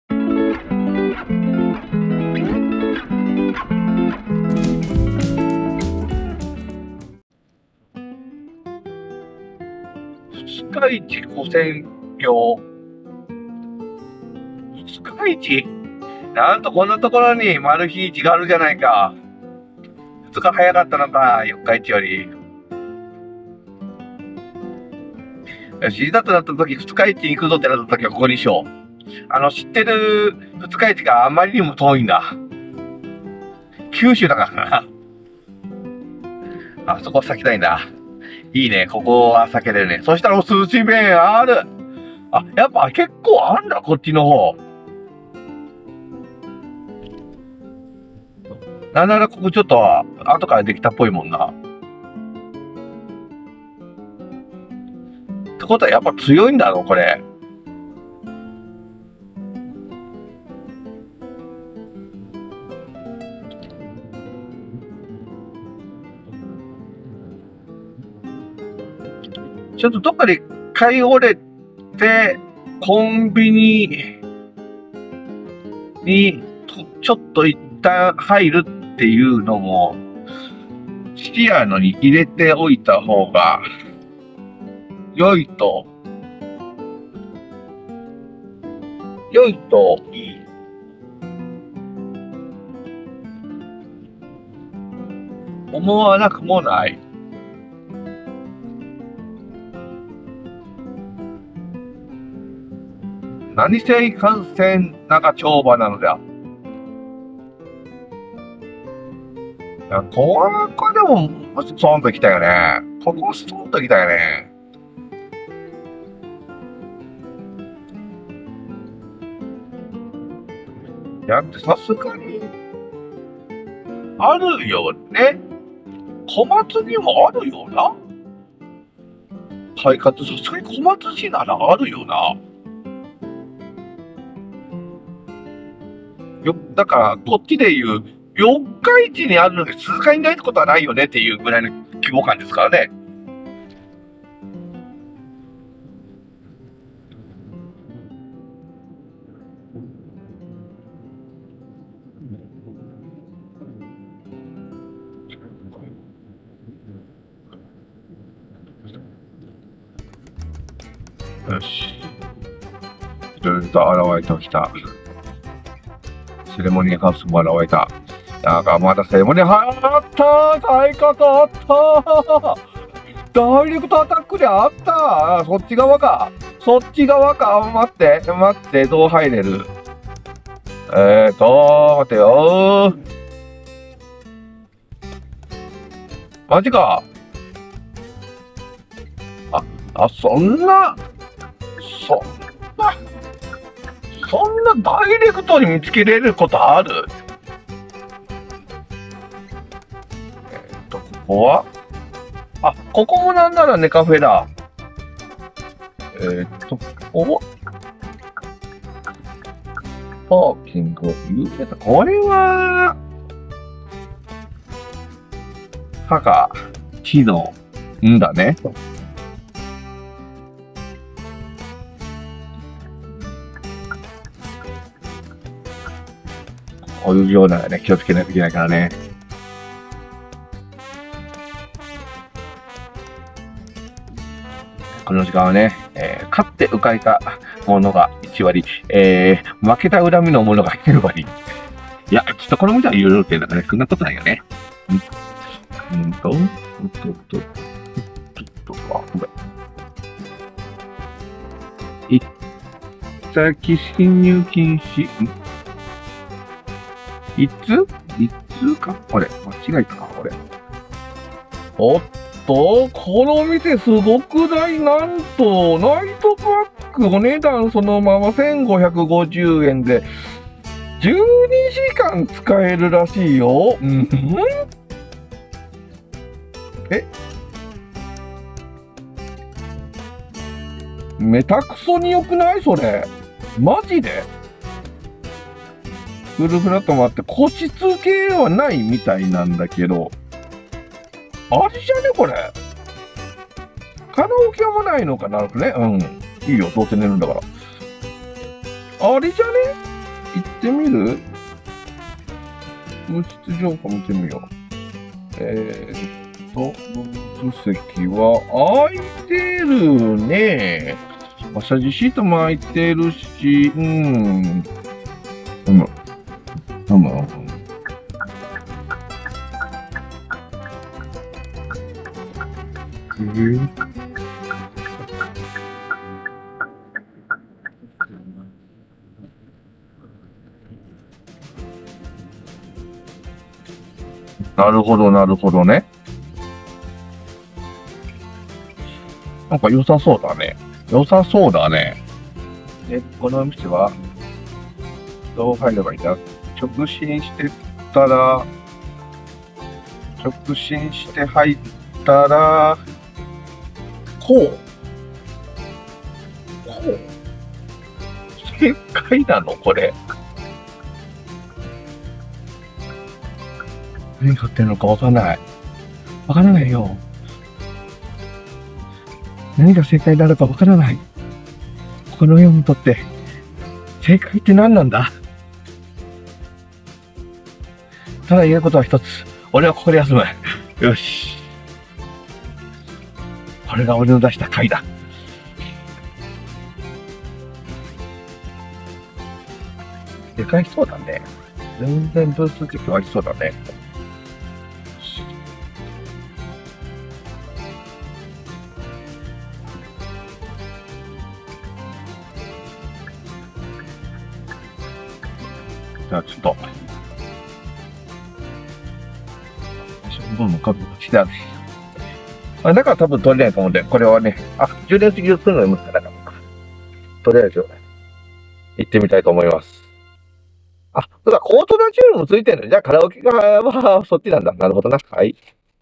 ST130 ep65 金沢へ向かってドライブ雑談Part10＠石川県野々市市【JR西日本ターミナル駅完全制覇の旅～北陸編～】
〒921-8801 石川県野々市市御経塚５丁目
国道8号/二日市跨線橋
5-chome, Okyozuka, Nonoichi, Ishikawa 921-8801
National Route 8/Futukaichi Overbridge